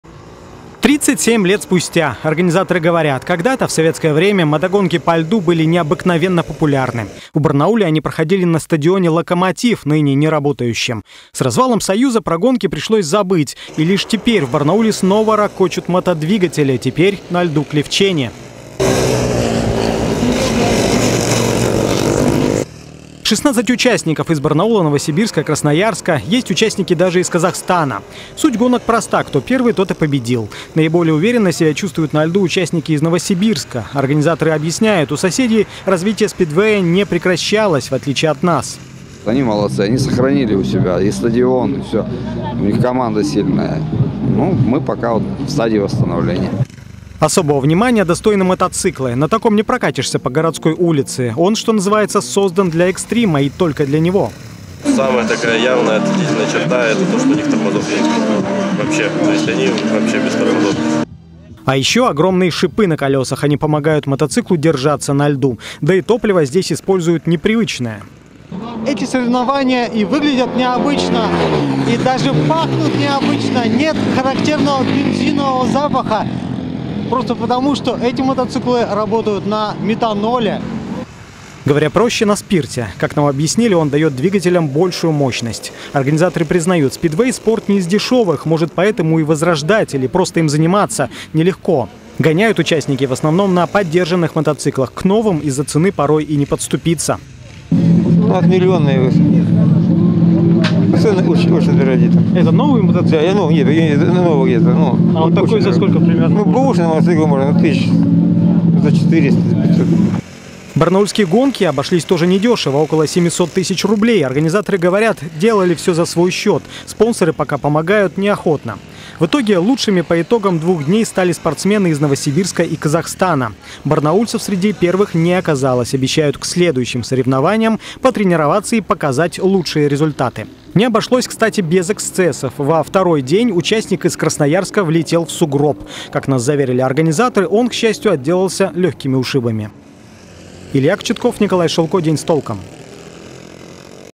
По льду без тормозов: во время мотогонок в Барнауле произошло ЧП
Рев двигателей, брызги льда, крутые виражи. В Барнауле спустя 37 лет впервые прошли краевые соревнования по мотогонкам на льду.